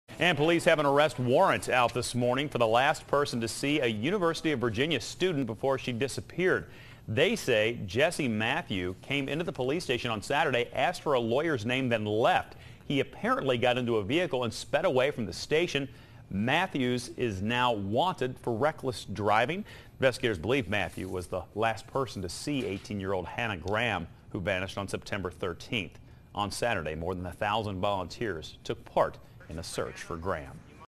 Man seen with UVA student faces driving charge
CHARLOTTESVILLE, Va. (AP) -- A man believed to be the last person seen with a University of Virginia student before she disappeared was being sought on arrest warrants charging him with reckless driving, a police chief said Sunday, adding authorities also want to ask him about the missing teen.

Virginia State Police have issued warrants for Jesse Matthew on a charge of misdemeanor reckless driving, Charlottesville Police Chief Timothy Longo said at a news conference Sunday. Matthew has not been charged in the disappearance of 18-year-old Hannah Graham, who was last seen early on Sept. 13 in Charlottesville.

Longo said police want to talk to Matthew, who was seen with Graham before she disappeared.

"I believe Jesse Matthew was the last person she was seen with before she vanished off the face of the Earth because it's been a week and we can't find her," Longo said.

"I've made no mistake about it. We want to talk to Jesse Matthew. We want to talk to him. We want to talk to him about his interaction with this sweet, young girl we can't find," Longo said.

Longo said Matthew had stopped by the Charlottesville Police Department's station on Saturday with several family members and asked for a lawyer. He was provided with a lawyer but left in a vehicle, driving at a high rate of speed that endangered other drivers, according to the police chief. He said Matthew was there for about an hour.

Virginia State Police spokeswoman said in an email late Sunday that State Police officers were conducing "surveillance" of the vehicle at the time but didn't pursue Matthew.

Police said they have focused on Graham's movements the night of Sept. 12 and into the early morning hours of Sept. 13. Graham, a sophomore from northern Virginia, met friends at a restaurant for dinner, stopped by two parties at off-campus housing units and left the second party alone, police have said.

Surveillance videos showed her walking, and at some points running, past a pub and a service station and then onto the Downtown Mall, a seven-block pedestrian strip lined with shops and restaurants.

"Somebody's gotta know where she is and we want to know who that person or persons are," Longo said.

"I don't want to get tunnel vision just because we have a name, just because we saw her with a particular person," he said.

Graham's parents appeared at the news conference and her father, John Graham, appealed for anyone with any information to call a police tip line.

"This is every parent's worst nightmare," John Graham said. "We need to find out what happened to Hannah and make sure it happens to no one else."

More than 1,000 volunteers participated in a weekend search for Hannah Graham, according to authorities.

Graham's disappearance has sent a ripple of fear through the quiet college town of Charlottesville. Students have said they've begun walking in pairs at night and are paying closer attention to their surroundings.

At least three other young women have disappeared in the area in the last five years, though police have said they do not think Graham's disappearance is linked to that of any of the other missing women.

The university president, Teresa A. Sullivan, issued a statement Sunday saying the university was committed to helping authorities in the search for the missing woman and "return her safely to her family."

"We are cooperating fully with law enforcement authorities as they continue their investigation," the emailed statement added.